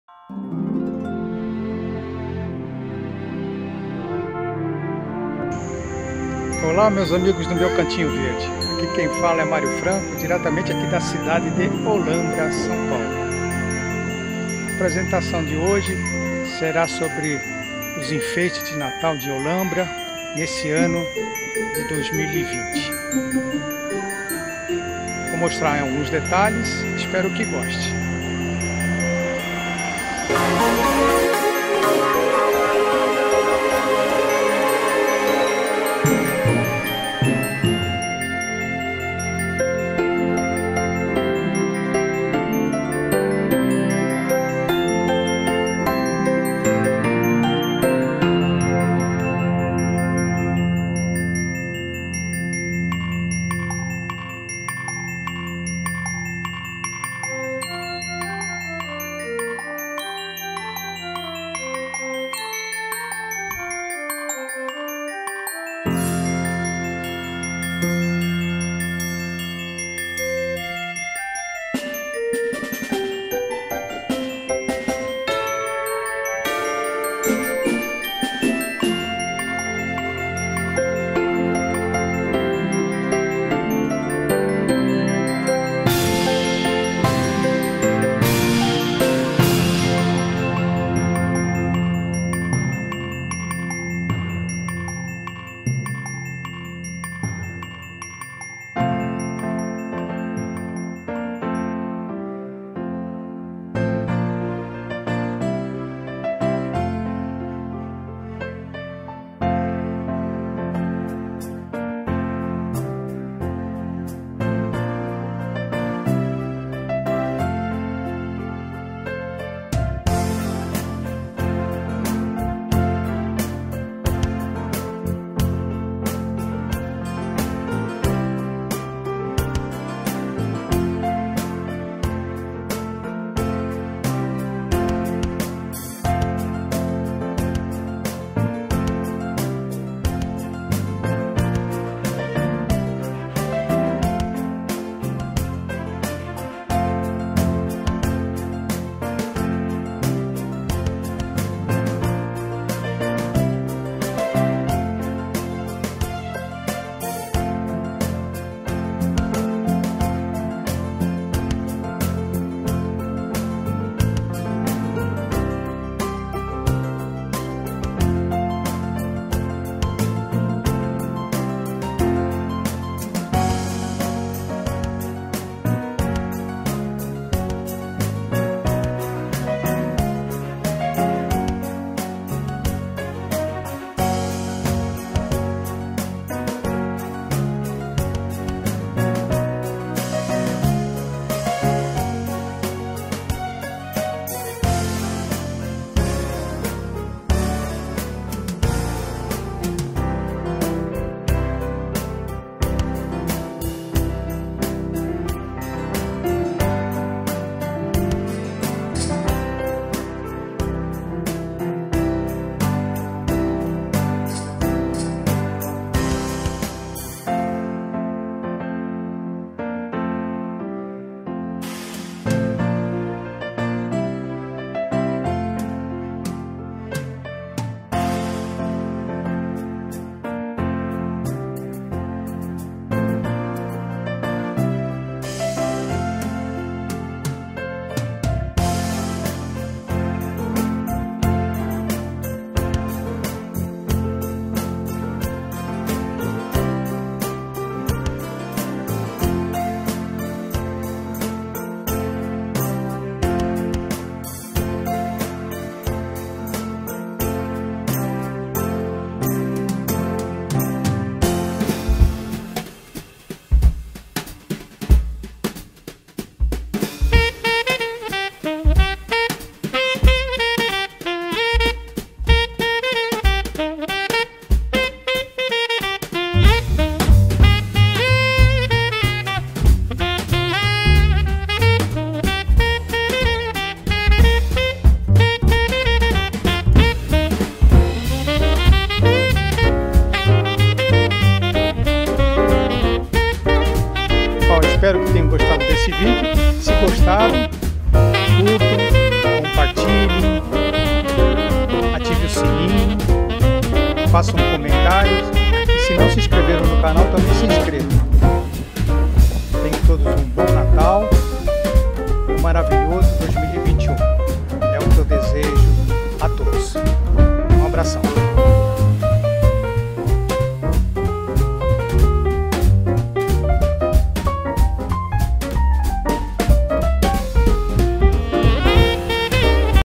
Natal 2020 - Holambra /SP
Vejam como está a decoração natalina da cidade de Holambra / SP esse ano.
O Moinho Povos Unidos localizado em uma das entradas da cidade está com uma iluminação espetacular